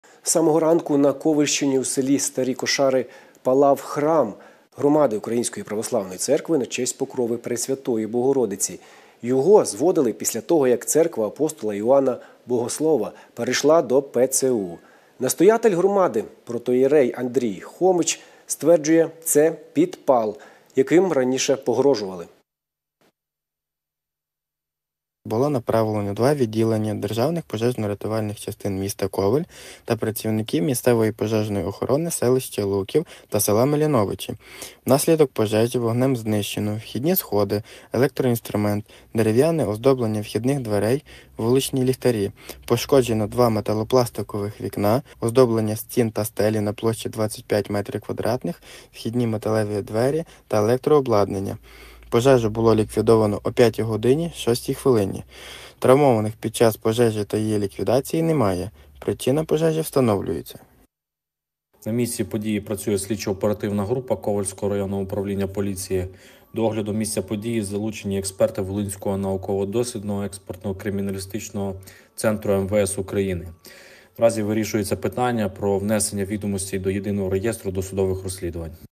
У Старих Кошарах на Ковельщині горіла церква, що будується
З самого ранку на Ковельщині у селі Старі Кошари палав храм громади Української Православної Церкви на честь Покрови Пресвятої Богородиці. Його зводили після того як церква апостола Іоанна Богослова перейшла до ПЦУ. Настоятель громади протоієрей Андрій Хомич стверджує – це підпал, яким раніше погрожували.